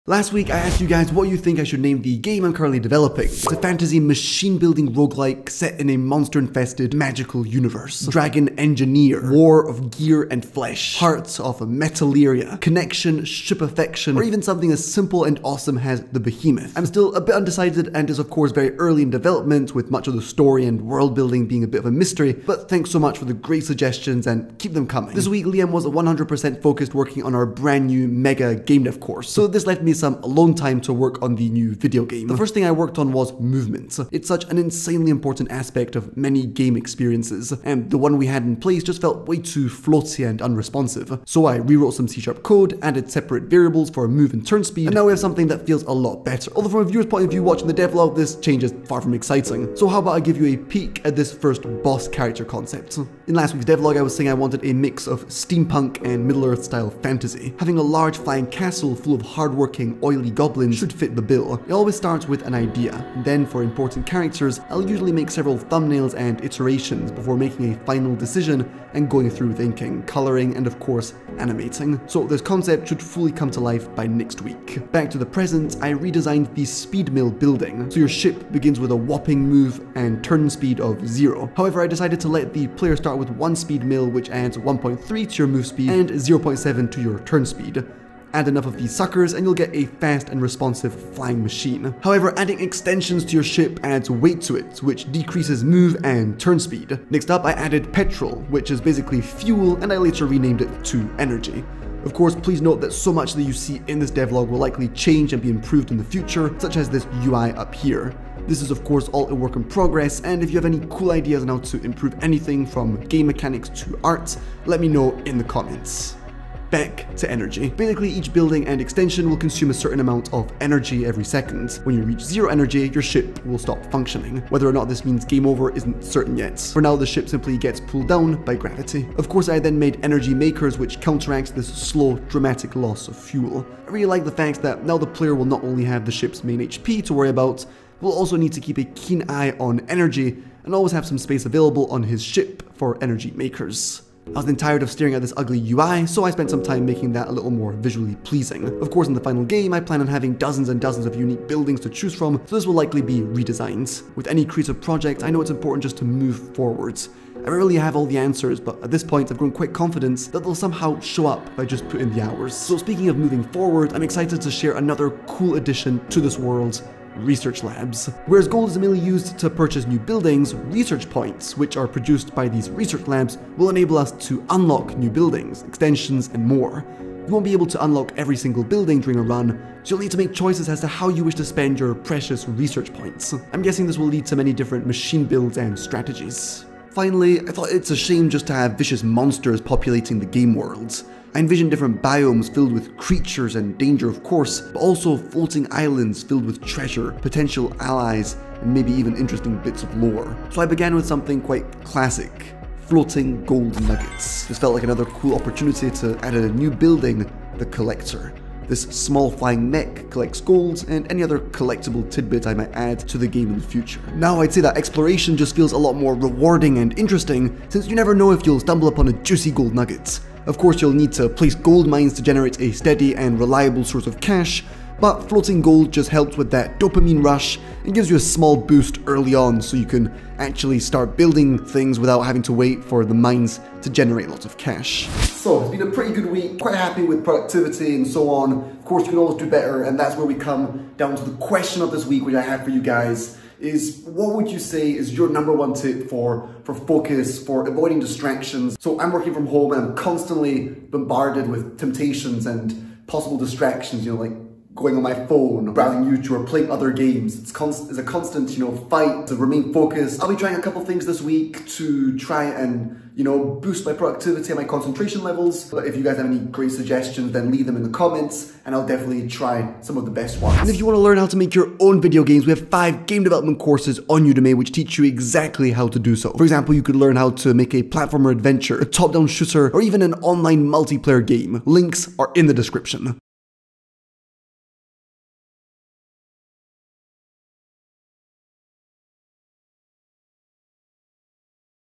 One Week of Indie Game Development
In this week of indie game dev, I continue making progress on our fantasy/steampunk war machine building game! I add new buildings, research labs, gold nuggets and begin brainstorming on boss character concepts!